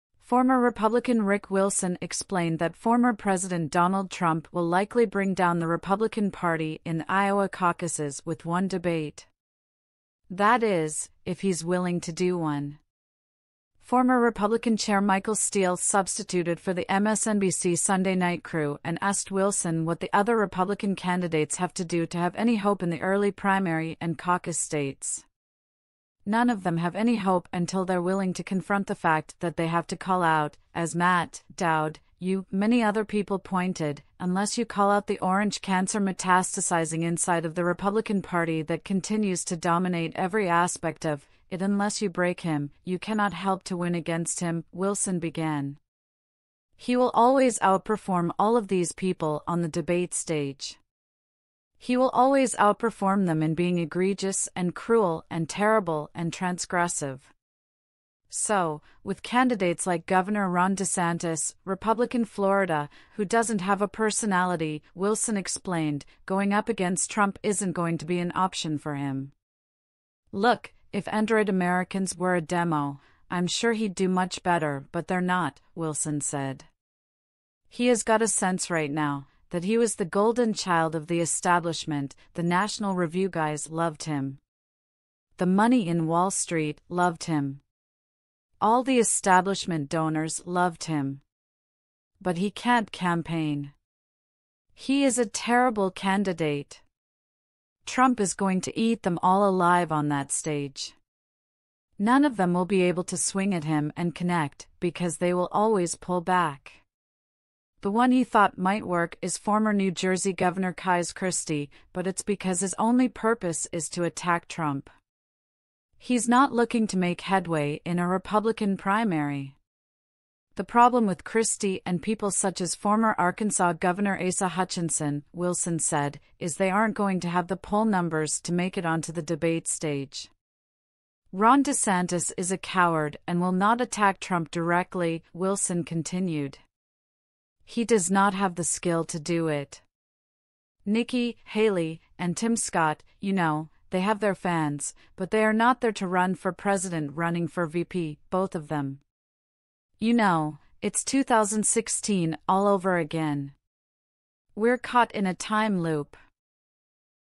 Ex-Republican explains why no one will survive a debate with Trump
Previous Conservative Rick Wilson made sense of that previous President Donald Trump will probably cut down the Conservative Alliance in the Iowa Gatherings with one discussion. That is, assuming he will do one.